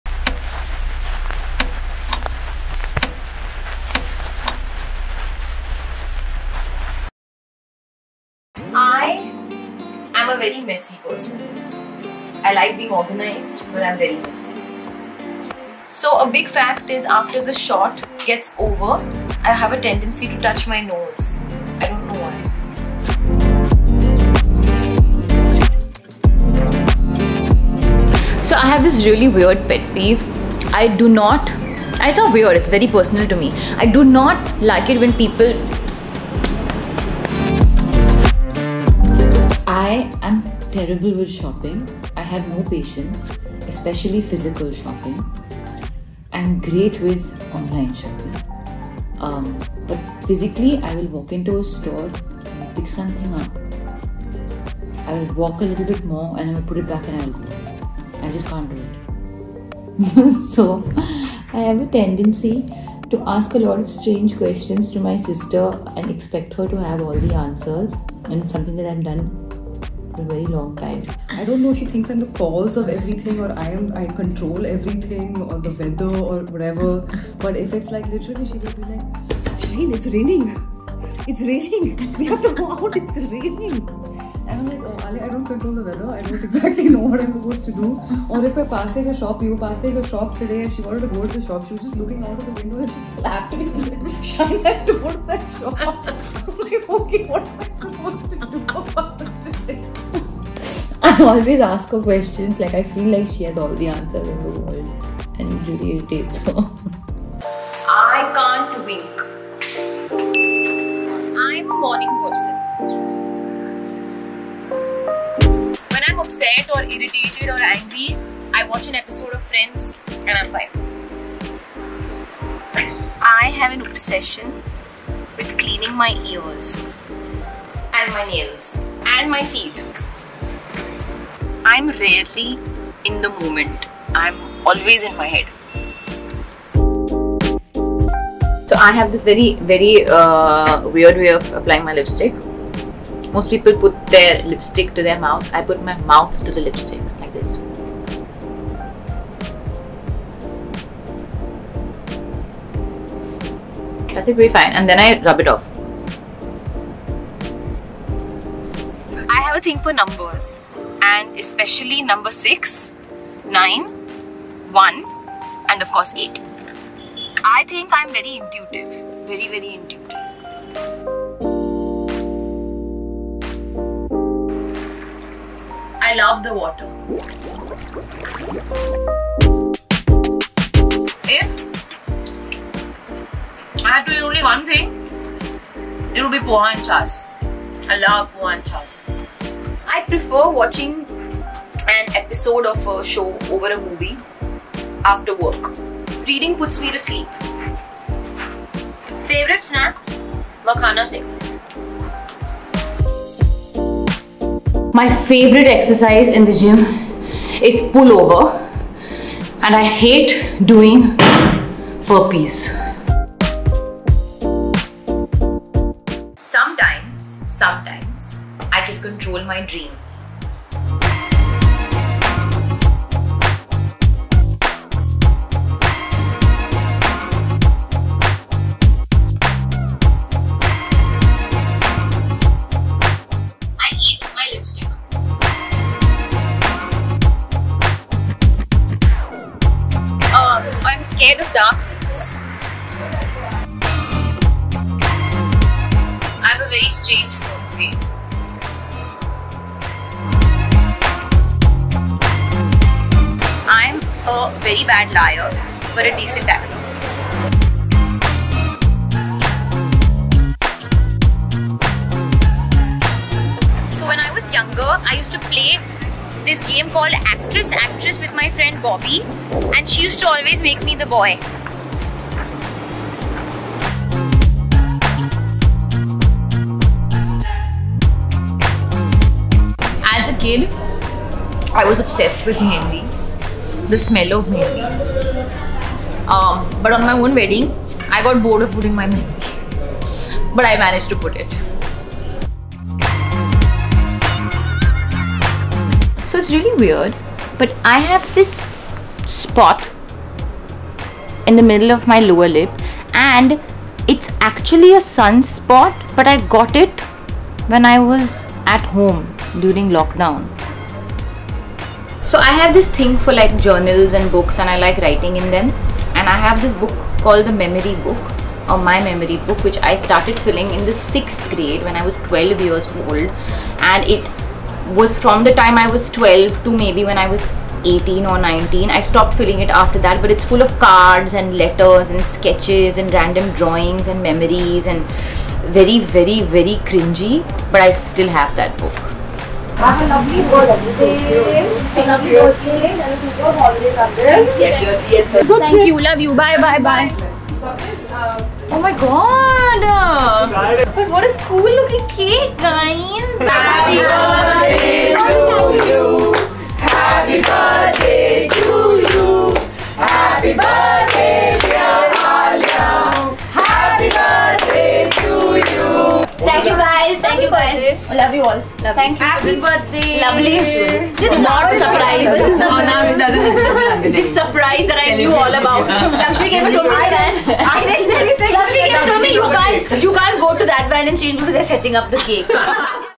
30 Facts As I turn 30 | Alia Bhatt
Hi guys! Welcome back to AliaBe.
As I turned 30  this year, I thought of sharing 30 things that
you all probably don’t know about me. And if you know any one of these 30 things then you probably know me really well!

Thank you for all the love and lots of love to you♥